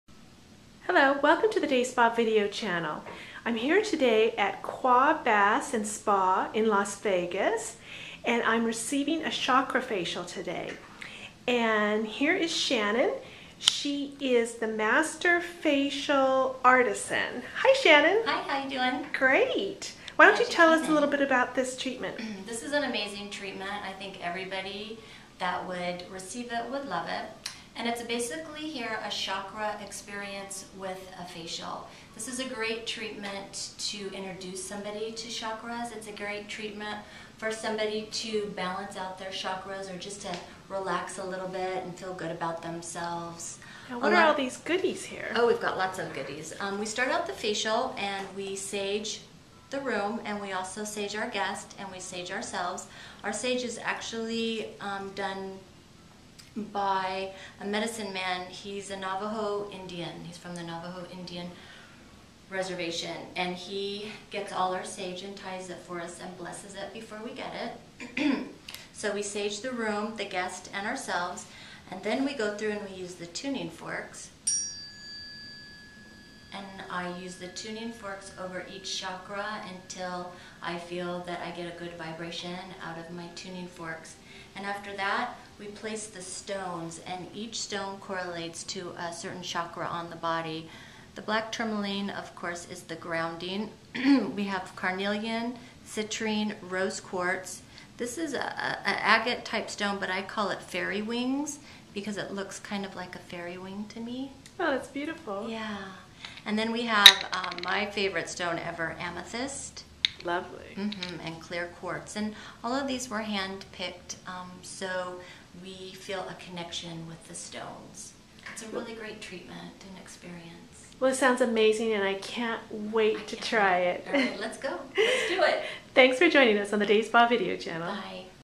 DAYSPA Video Channel: Qua Baths & Spa
The balancing Chakra Facial at Qua Baths & Spa in Las Vegas uses blessed sage, tuning forks and special stones to enhance the guest experience.